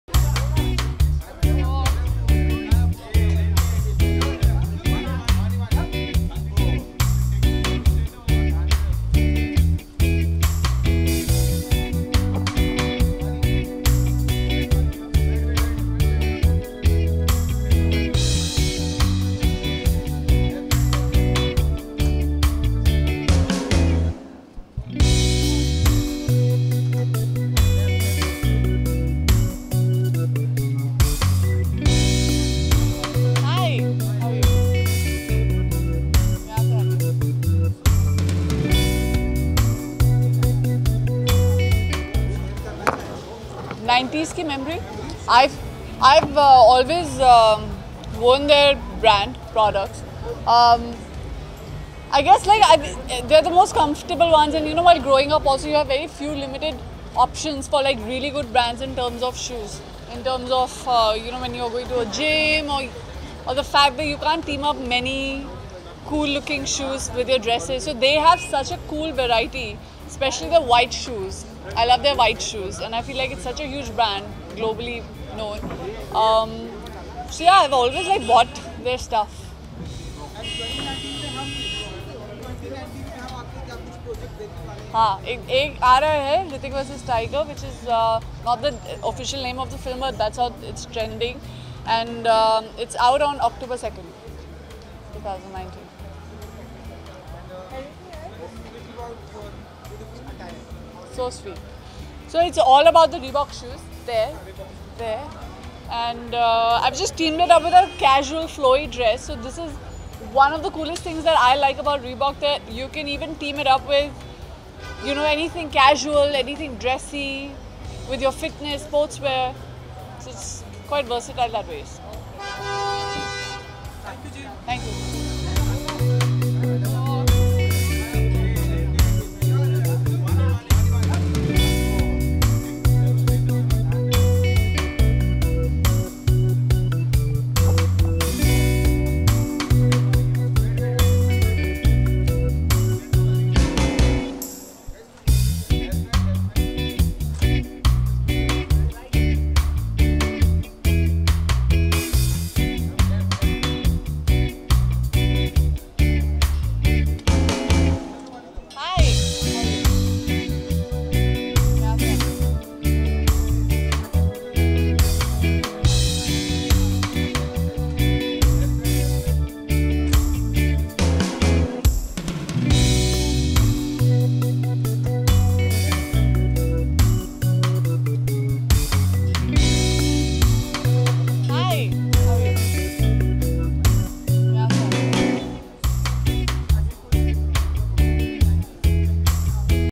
Vani Kapoor look fabulous at Reebok Classic Aztrek event ; Watch Video | FilmiBeat
Vani Kapoor look fabulous at Reebok Classic Aztrek event.While the Befikre actress Vani Kapoor has not made much  headway in her  career elsewhere at Yashraj films she continues  to be regarded as a  potential  star.  Aditya Chopra who launched  Vani in  Shuddh Desi Romance  as  the second lead  quickly saw her potential and  re-launched her as  the lead  in the high-profile Parisian romance  Befikre which  bombed  miserably at the boxoffice.Recently  Vani Kapoor spotted in gorgeous look at the Reebok Classic Aztrek event.